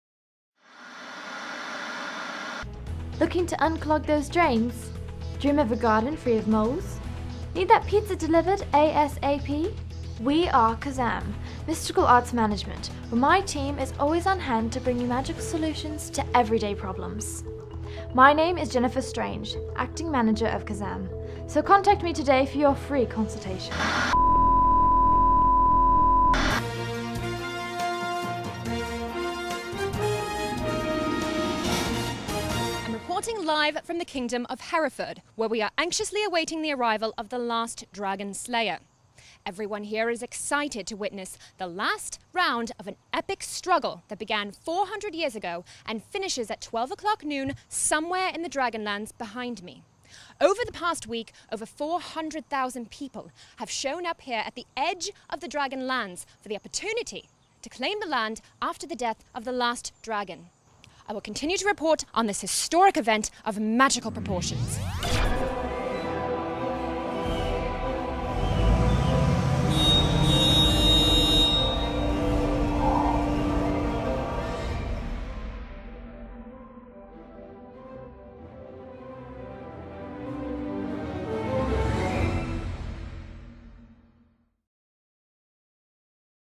The Last Dragonslayer by Jasper Fforde Book Trailer
Jennifer Strange runs Kazam, a struggling employment agency for magicians. Suddenly, the visions start, predicting the death of the world's last dragon at the hands of a mysterious warrior. If the visions are true, her entire world could change. Because something is coming. Something known as . Big Magic. and find out more about the book YA/Childrens/Fantasy